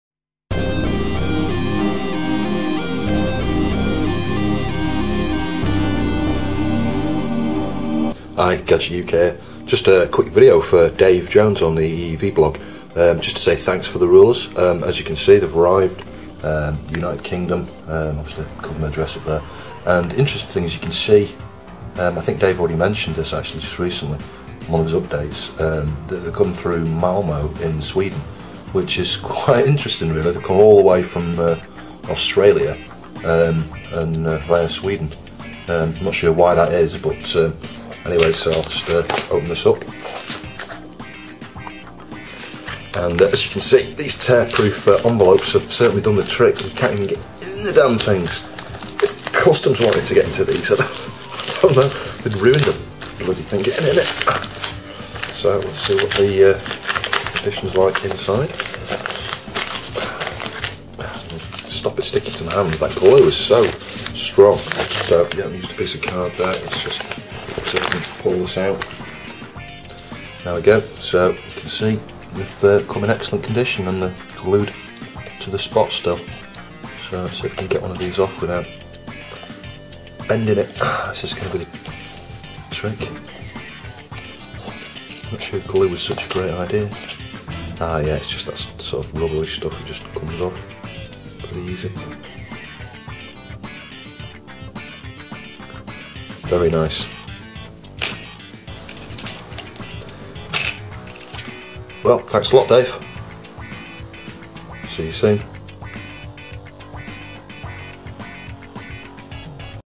EEVBlog uRulers Receipt
A quick video showing the uRulers I received today from Dave Jones.  Thanks Dave, they are arriving in the UK now.